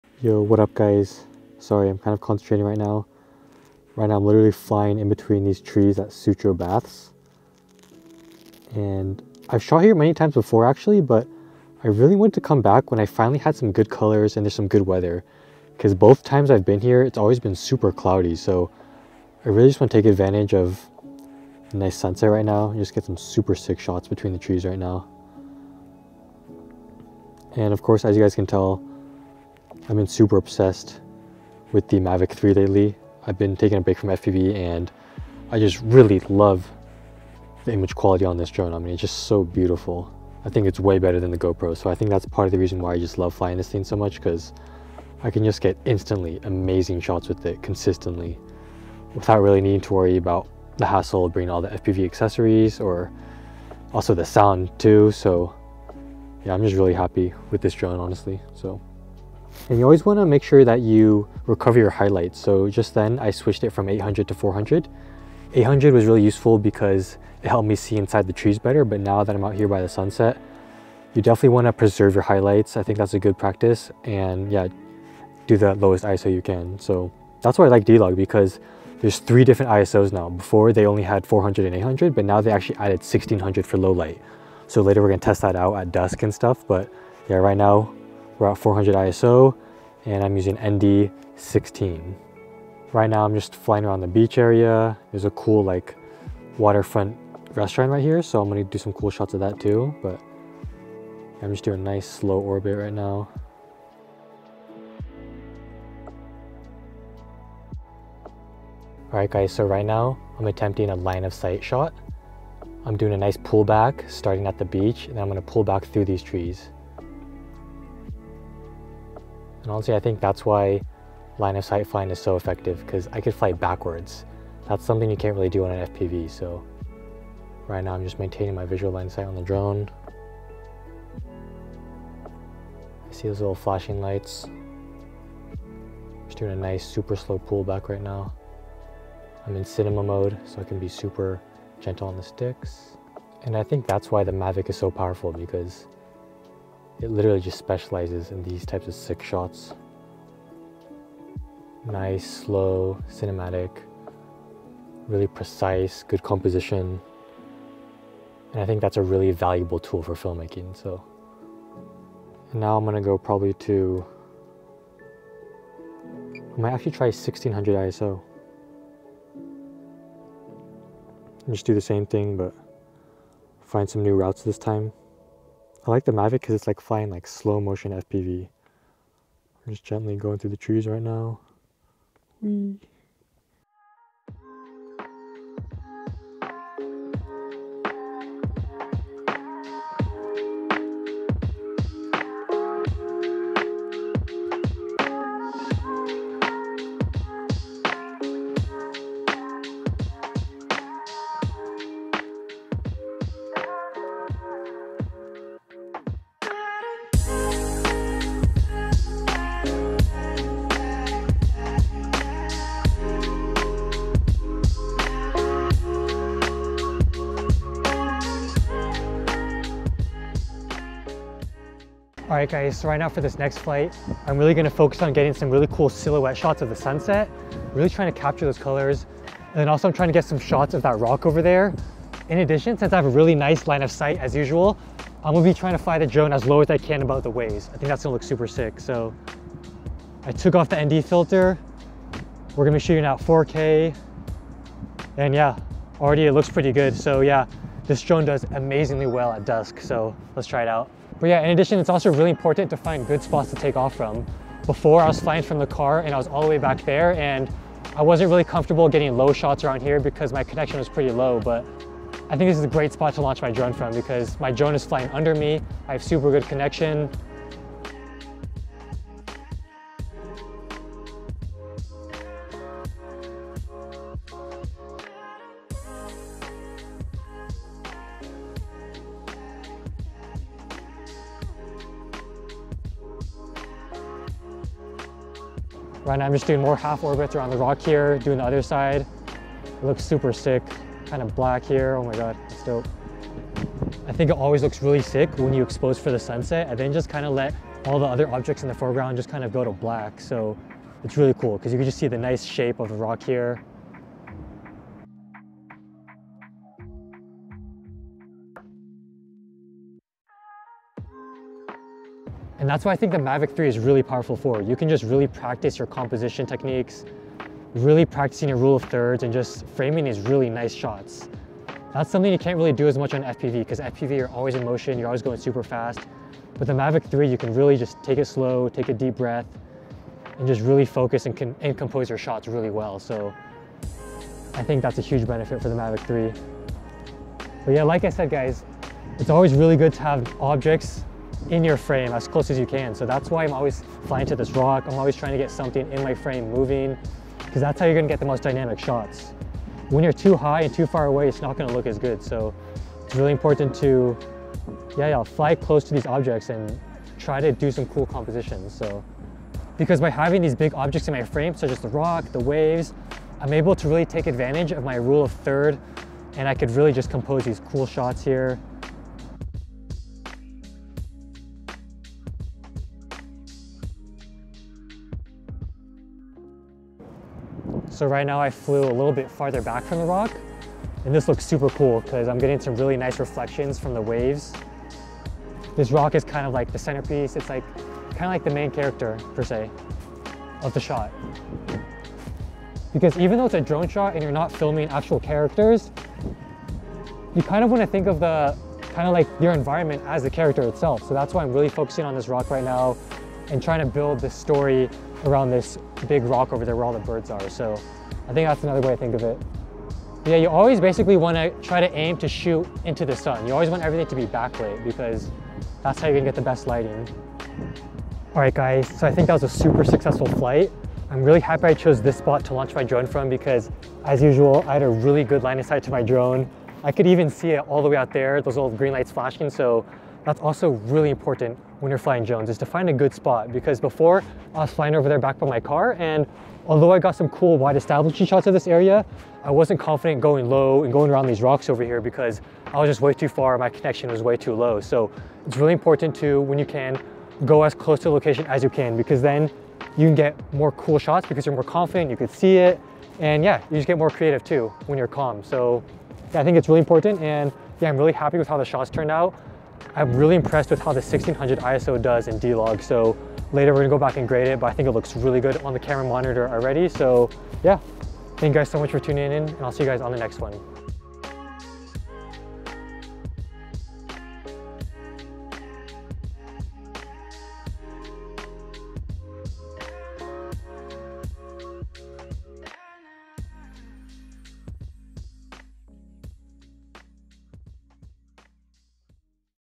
How does the DJI Mavic 3 Perform at Lowlight?
In today's video we are putting the DJI Mavic 3 to the test to see how it performs in lowlight. I will be shooting at Cinema 4k DCI in D Log 24 fps. I think it performs absolutely amazing at Dusk and is super easy to grade later in Davinci Resolve.